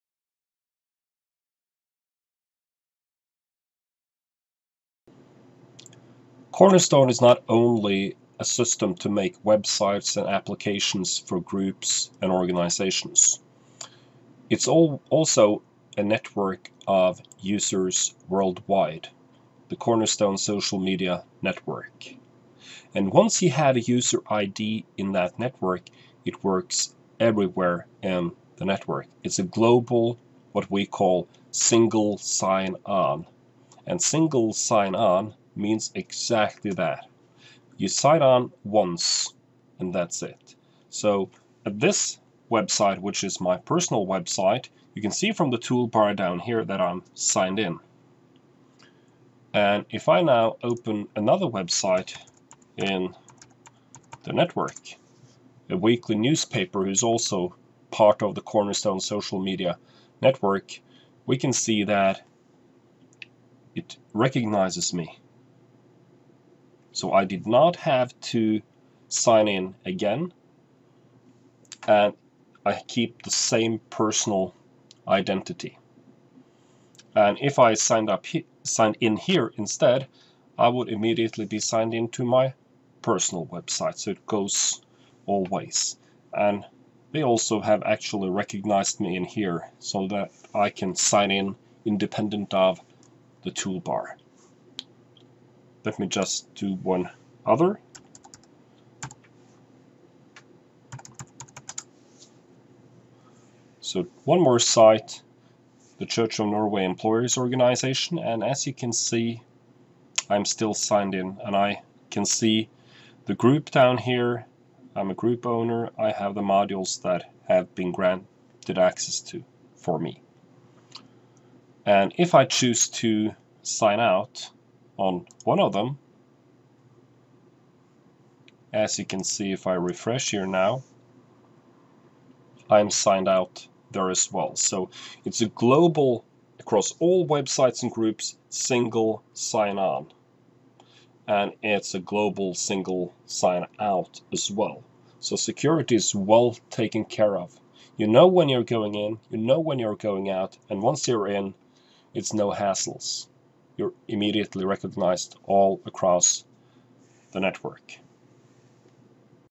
Single-sign-on explained
This video explains the single-sign-on capabilities of the Cornerstone Social Media Network